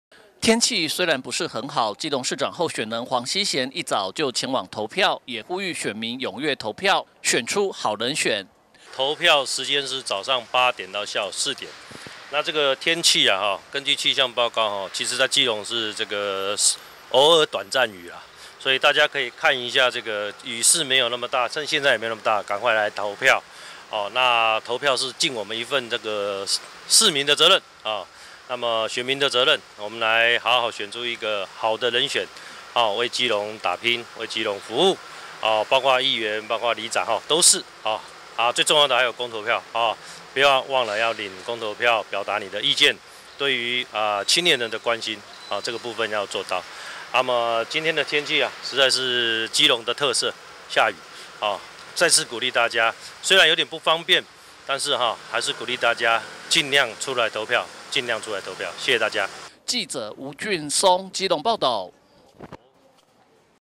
新聞 基隆|基隆市長候選人黃希賢|投票|黃希賢投票 呼籲選民踴躍投票 選出好人選
黃希賢投票 呼籲選民踴躍投票 選出好人選

(中嘉吉隆新聞記者吳俊松/基隆報導)天氣雖然不是很好，基隆市長候選人黃希賢，一早就前往投票，也呼籲選民踴躍投票，選出好人選！